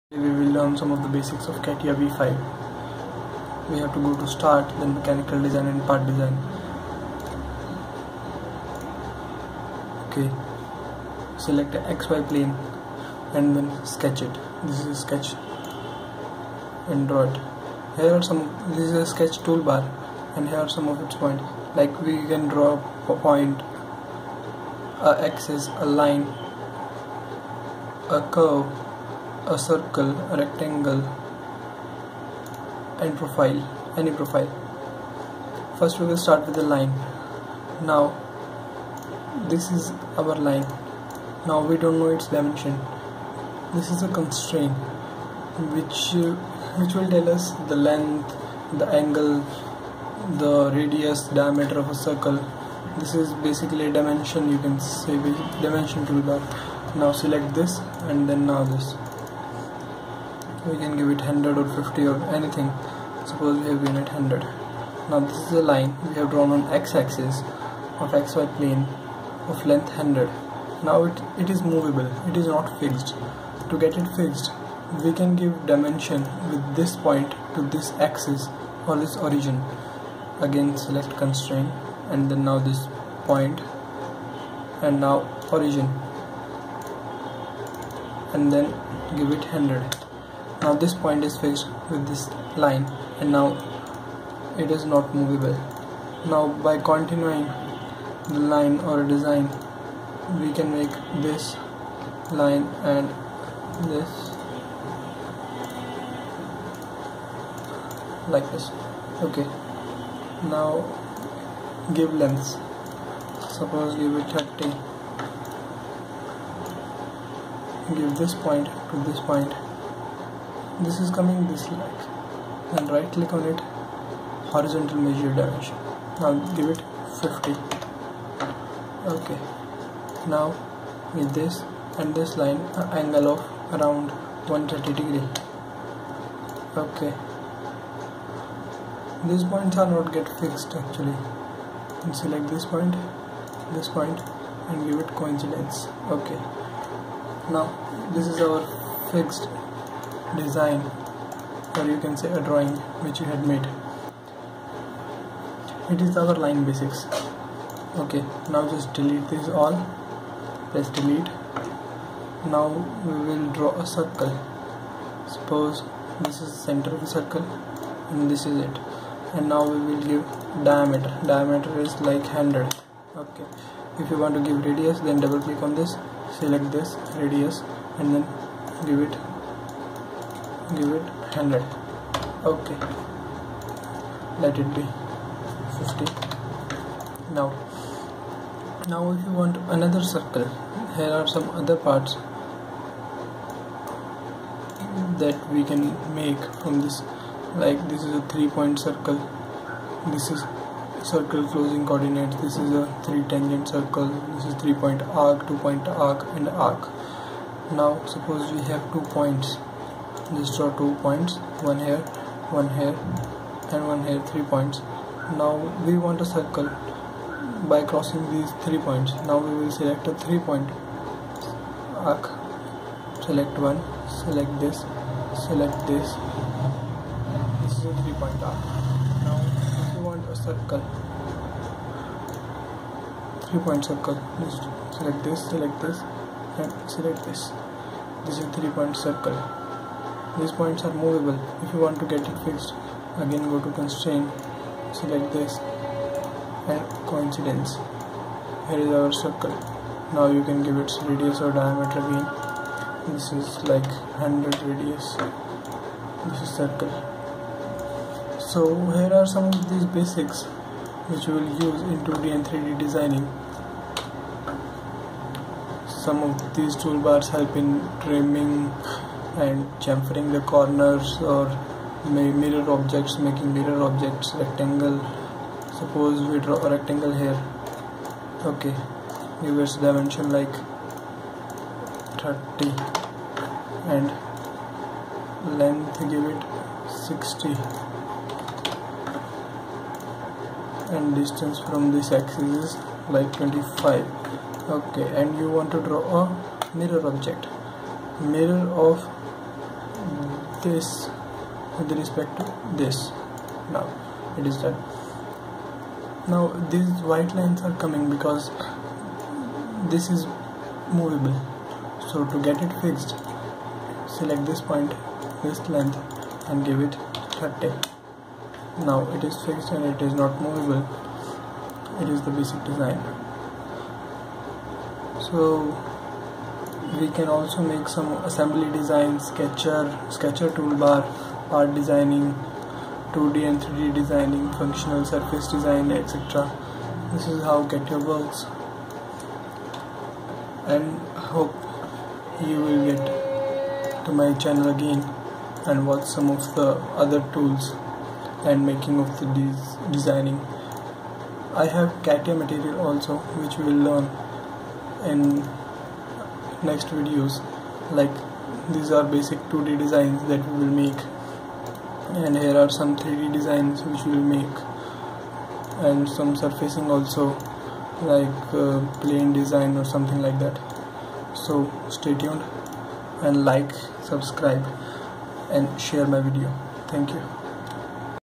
Learn Catia v5 Basics [ From Starting] -01
This vedio is all about the basis of Catia V5. We will learn how to draw some simple 2d and 3d designs. Catia V5 is a designing software used by many aerospace and automobile companies these days.
We will learn use constraint draw a simple sketches and using mirror etc..